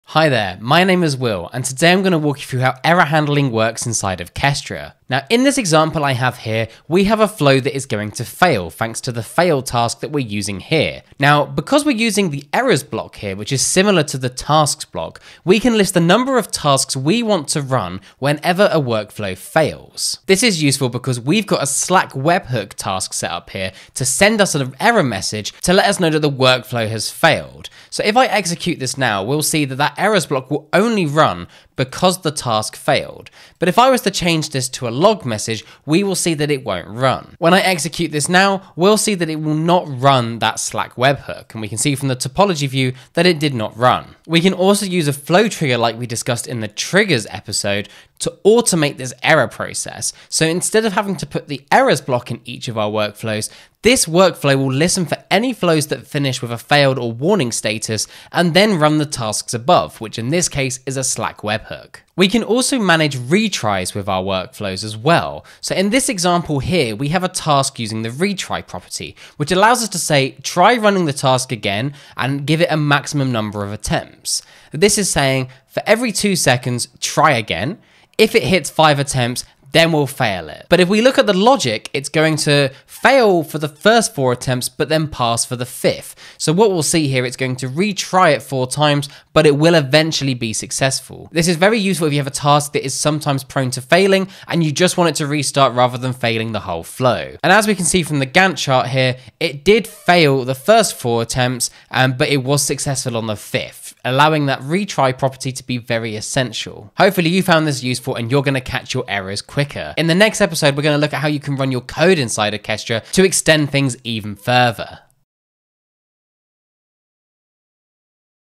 Handle Errors and Failures | Kestra Tutorial 2025
Will guides you through the essentials of error handling and retries in Kestra. Discover how to configure your workflows to manage failures smoothly with error blocks and notifications, such as Slack alerts. Learn how to set up automatic retries for robust workflow execution.